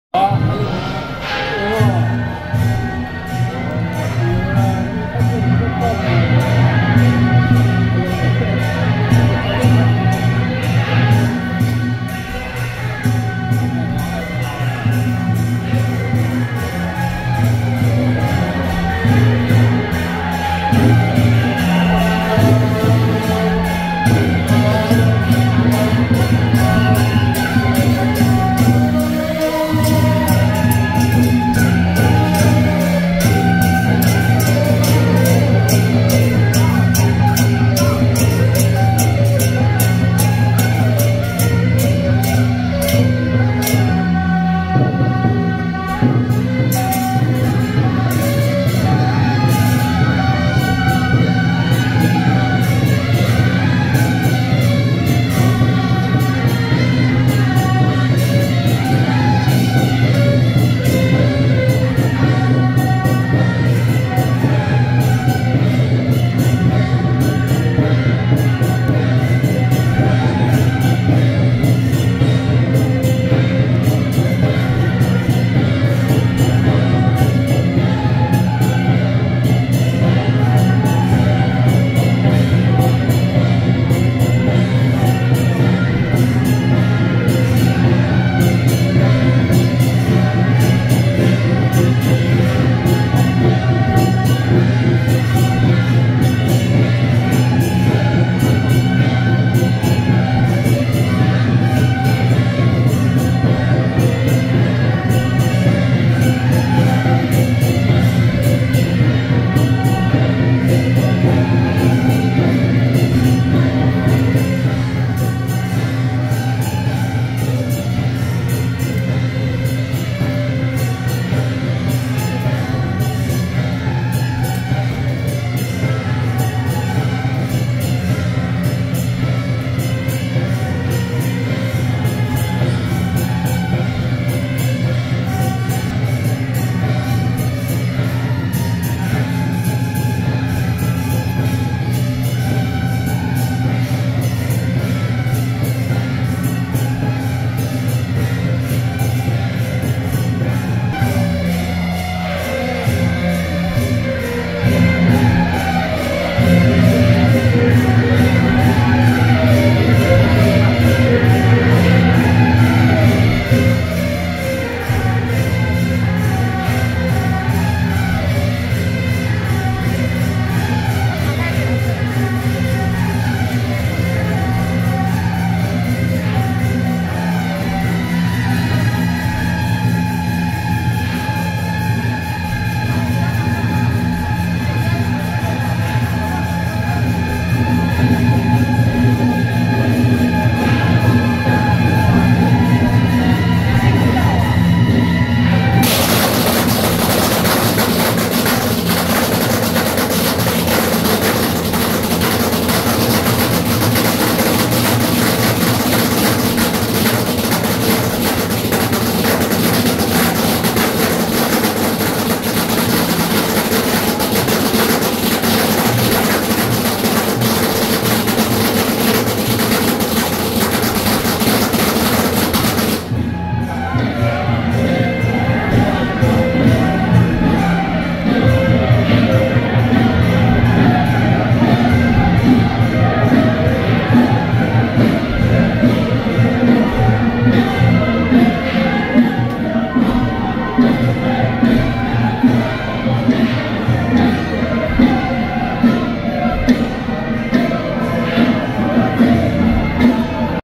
【新竹龍舞社-大鼓弄＆新竹尊新堂&大甲赫武堂-邢府千歲 聖駕】~112新竹都城隍廟奉旨遶境賑孤(竹塹中元城隍祭)
新竹龍舞社-大鼓弄＆新竹尊新堂&大甲赫武堂-邢府千歲 聖駕